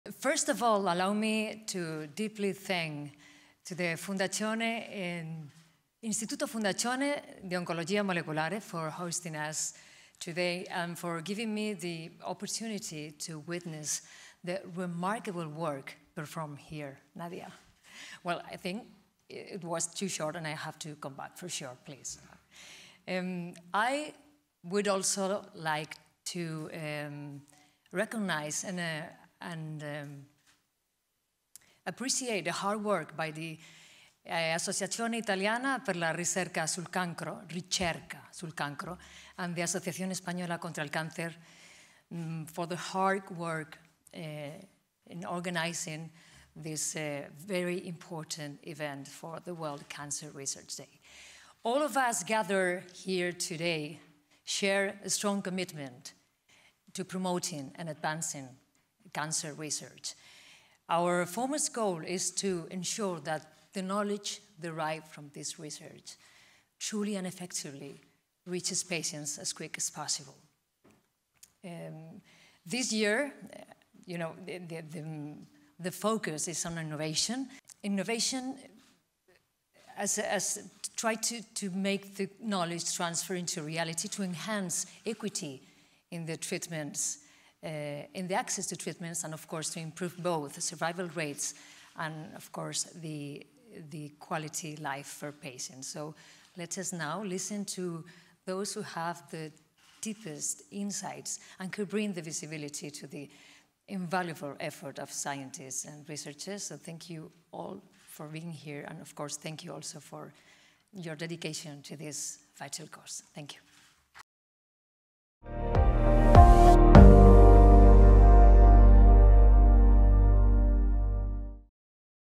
Palabras de S.M. la Reina en el Acto Internacional del “Día Mundial de la Investigación en Cáncer”
Good morning, buongiorno.

First of all, allow me to deeply thank the Fondazione, Istituto Fondazione di Oncologia Molecolare for hosting us today and for giving me the opportunity to witness the remarkable work performed here. Well, I think it was crucial, and I have to come back.

I would also like to recognise and appreciate the hard work by the Associazione Italiana per la Ricerca sul Cancro and the Asociación Española contra el Cancer for the hard work in organising this very important event for the world cancer research. All of us gathered here today share a strong commitment to promoting and advancing cancer research.

Our foremost goal is to ensure that the knowledge derived from this research truly and effectively reaches patients as quickly as possible. This year, you know, the focus is on innovation. Innovation is trying to make the knowledge transfer into reality to enhance equity in the access to treatments and, of course, to improve both the survival rates and, of course, the quality of life for patients.

So let us now listen to those who have the deepest insights and could bring the visibility to the invaluable effort of scientists and researchers.

Thank you all for being here. And, of course, thank you also for your dedication to this.

TRADUCCIÓN

Buenos días, buongiorno.

En primer lugar, permítanme agradecer profundamente a la Fundazione, Istituto Fondazione di Oncologia Molecolare por recibirnos hoy y por darme la oportunidad de presenciar el extraordinario trabajo realizado aquí. Bueno, creo que fue crucial y tengo que volver.

También me gustaría reconocer y agradecer el arduo trabajo de la Associazione Italiana per la Ricerca sul Cancro y la Asociación Española contra el Cáncer por el arduo trabajo en la organización de este evento tan importante para la investigación mundial del cáncer. Todos los reunidos hoy aquí compartimos un fuerte compromiso con la promoción y el avance de la investigación sobre el cáncer.

Nuestro principal objetivo es garantizar que el conocimiento derivado de esta investigación llegue verdadera y eficazmente a los pacientes lo más rápido posible. Este año, ya sabéis, la atención se centra en la innovación. La innovación es intentar hacer realidad la transferencia de conocimiento para mejorar la equidad en el acceso a los tratamientos y, por supuesto, mejorar tanto las tasas de supervivencia como, por supuesto, la calidad de vida de los pacientes.

Así que escuchemos ahora a aquellos que tienen los conocimientos más profundos y podrían dar visibilidad al inestimable esfuerzo de los científicos e investigadores.

Gracias a todos por estar aquí. Y, por supuesto, gracias también por vuestra dedicación a esto.

Gracias.